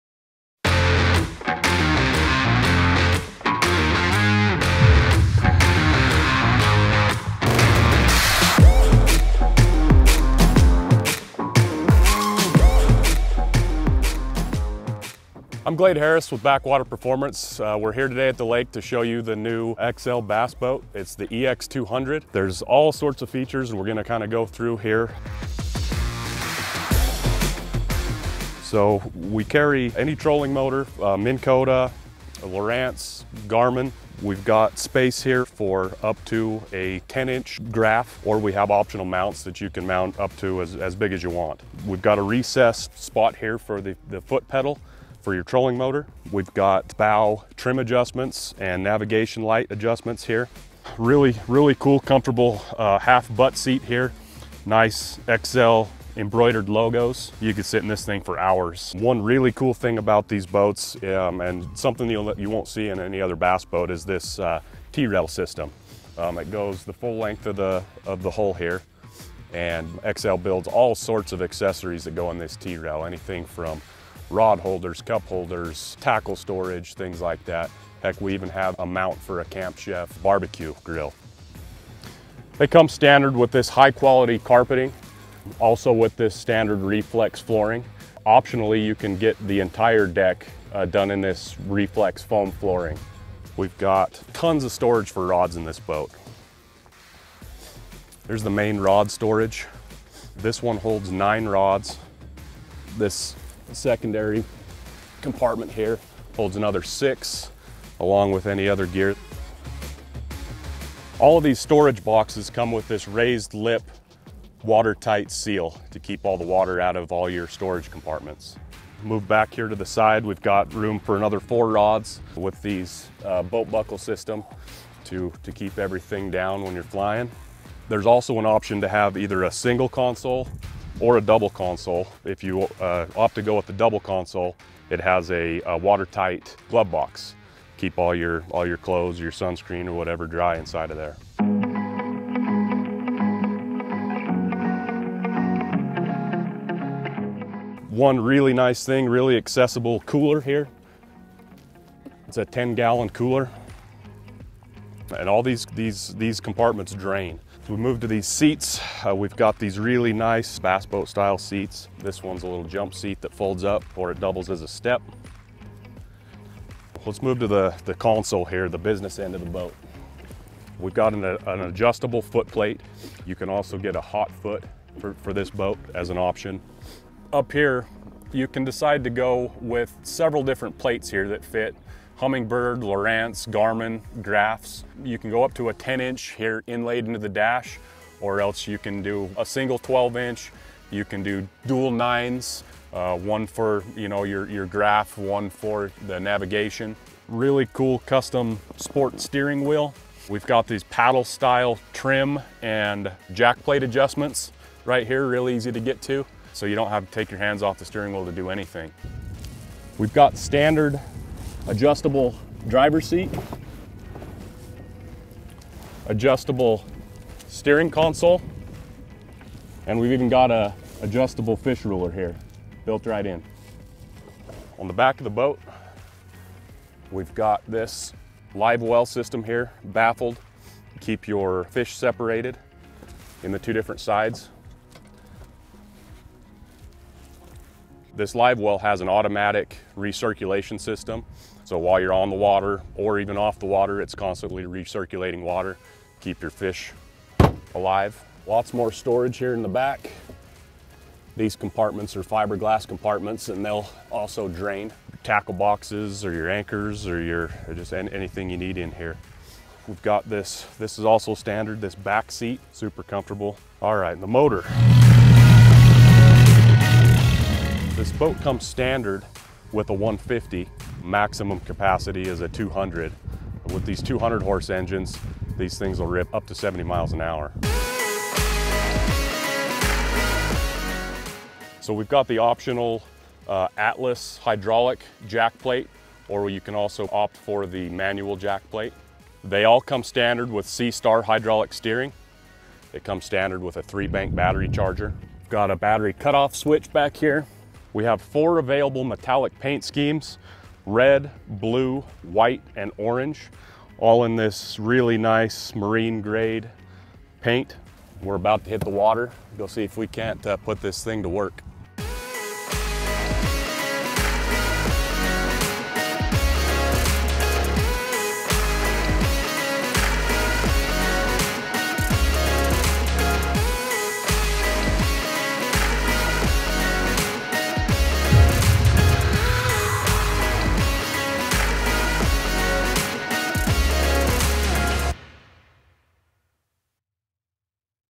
Best Aluminum Bass Boat - 2024 Excel EX 200 Bass Boat Full Walkthrough
Check out the new Excel EX 200 Bass Boat.  This exciting new full feature aluminum bass boat blows away the competition with top to bottom quality, fit and finish and loaded tournament ready. Up to 200 horsepower and speeds over 60 mph.